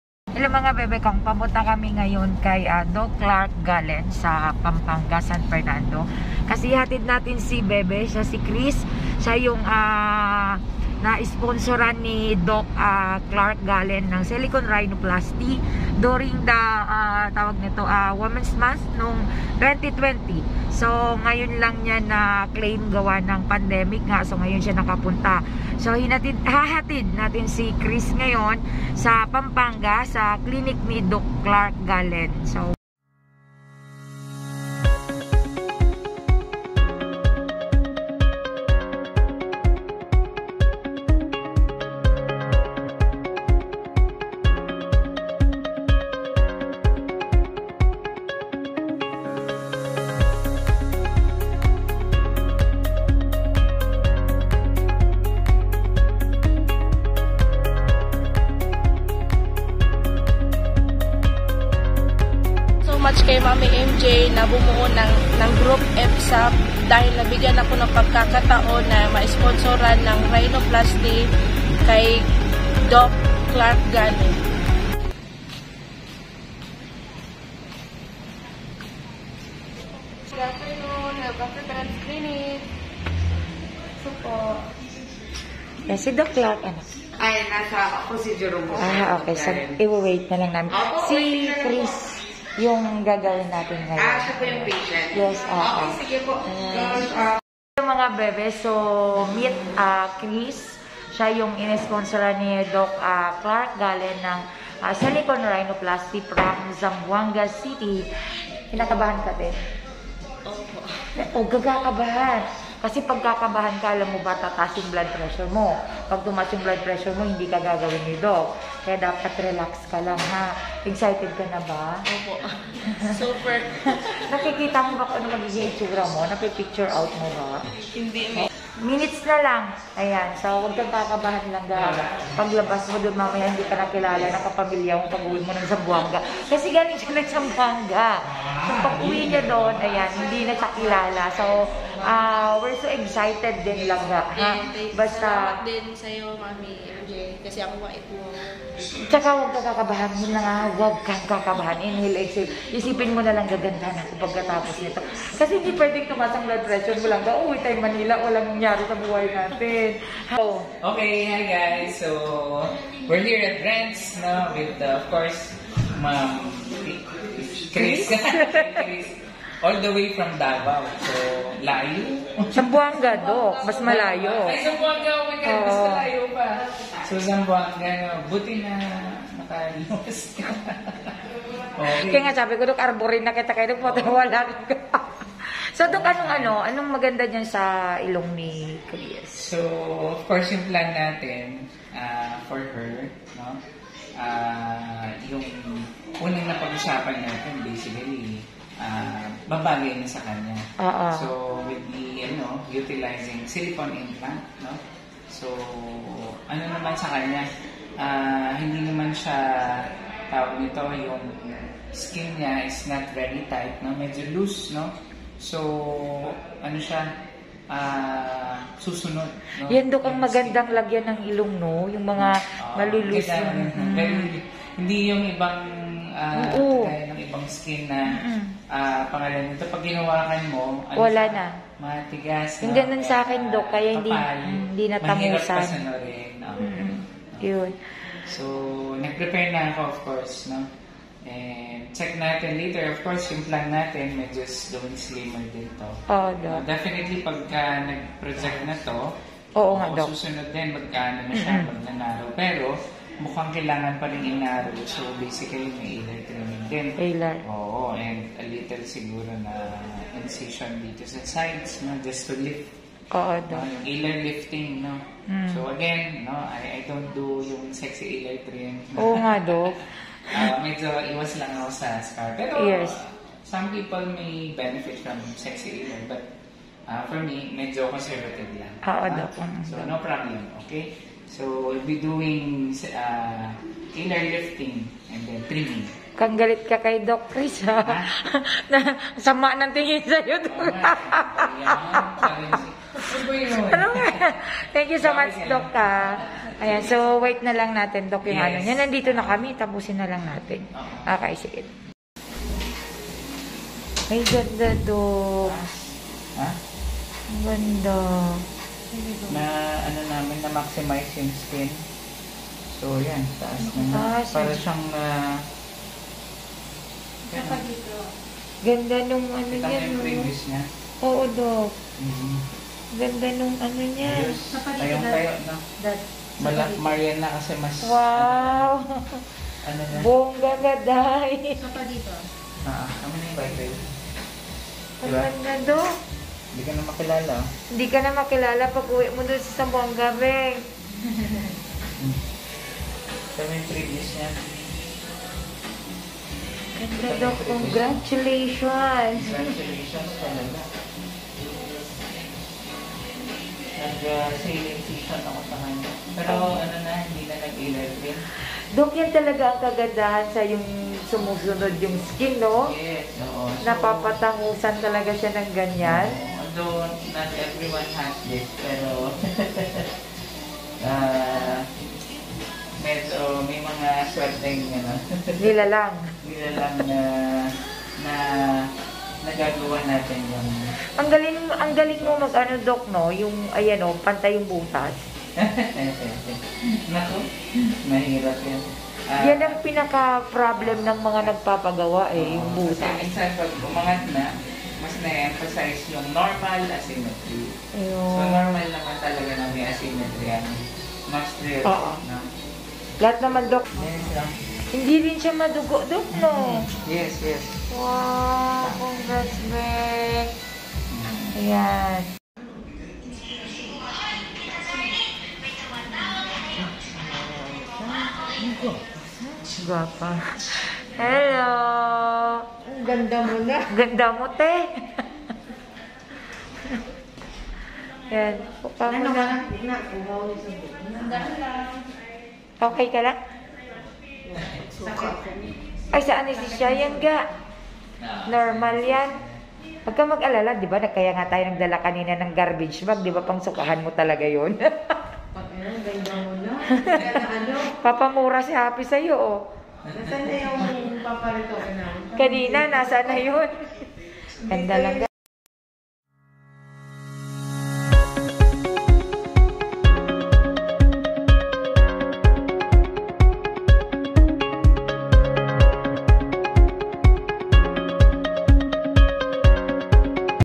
Babaeng Nagkaroon Ng Bagong Buhay sa tulong ng Isang Doctor/ Doc Clark Galen ENT/Cosmetic Surgeon
thank you so much Doc Clark Galen and to EPSAPP family. Mission Accomplished.than you so much